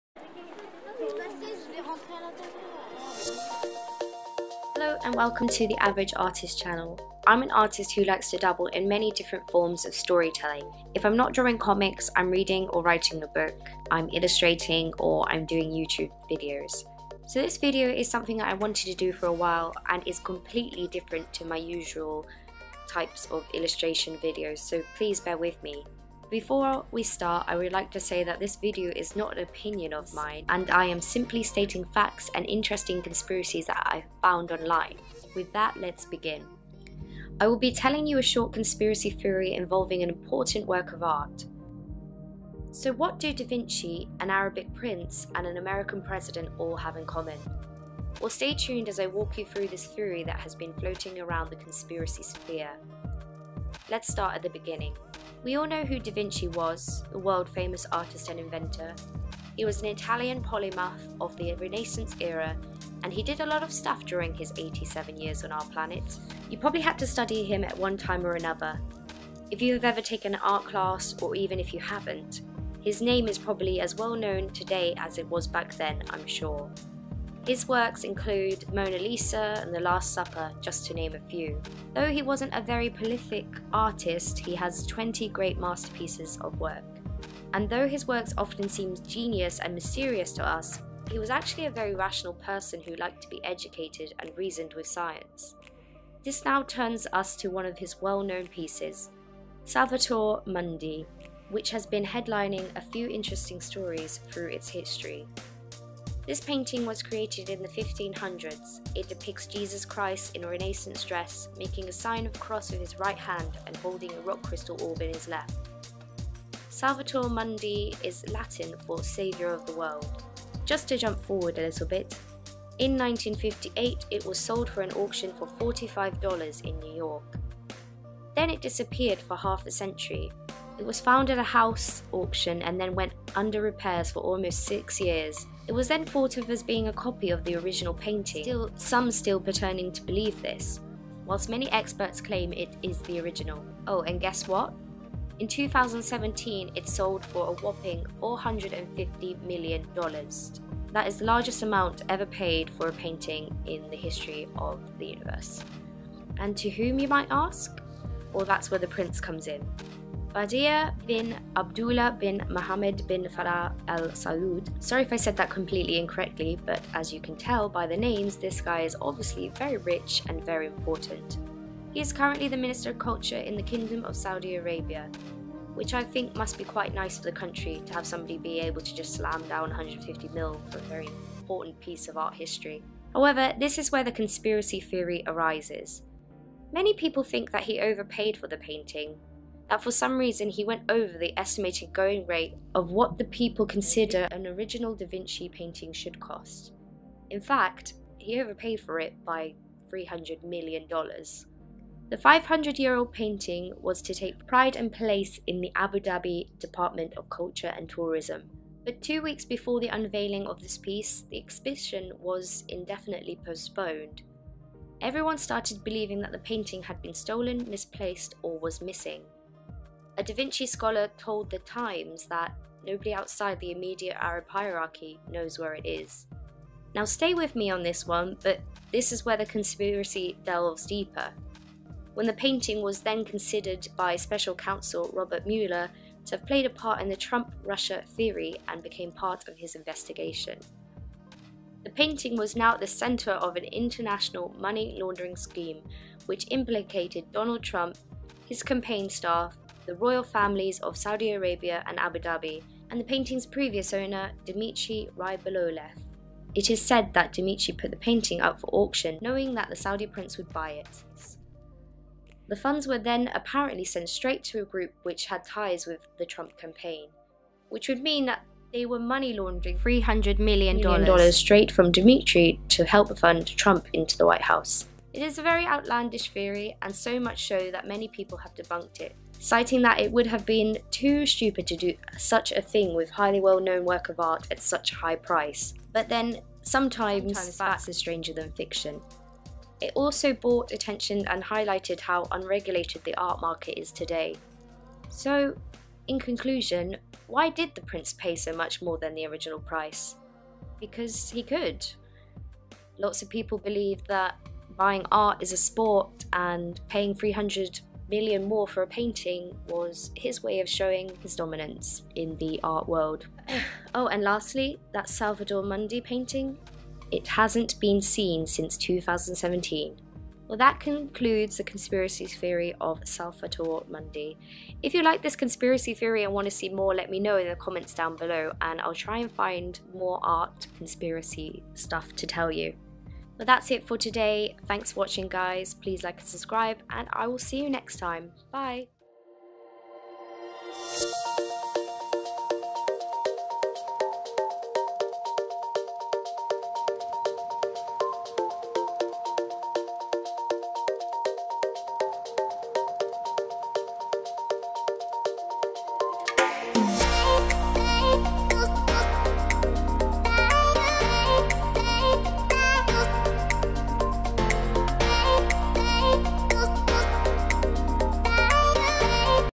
Conspiracy of the Paintings
Trying to make this a series. Of repainting classical paintings, I think it's good for development too! This one delves into the conspiracy behind the painting whilst I try to repaint it. I found it so interesting to try to paint The Salvator Mundi which sold for a whopping 450 MILLION, and it currently purposed to be missing (?).

Category

Licence